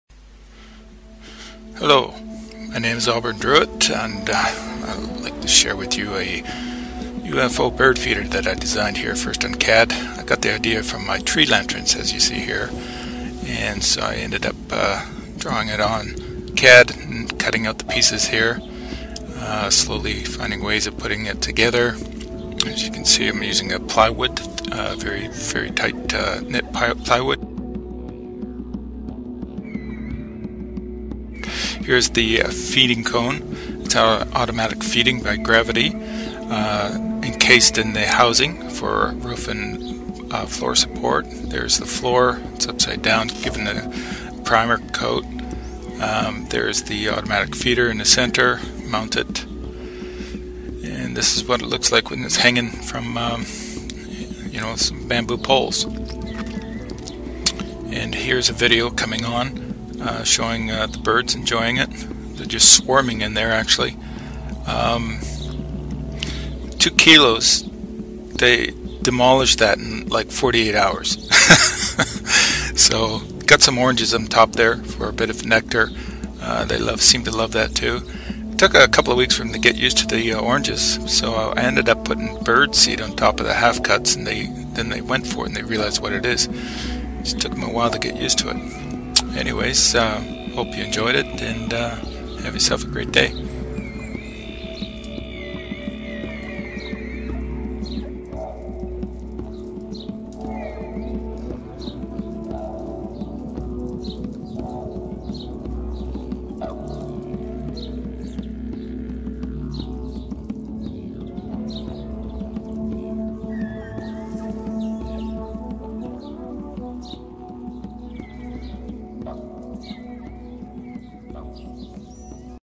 UFO Bird feeder
Howdy Here's my design of a auto bird feeder. Just fell it up with wild bird feed and watch them fly in and out like happy chappies:)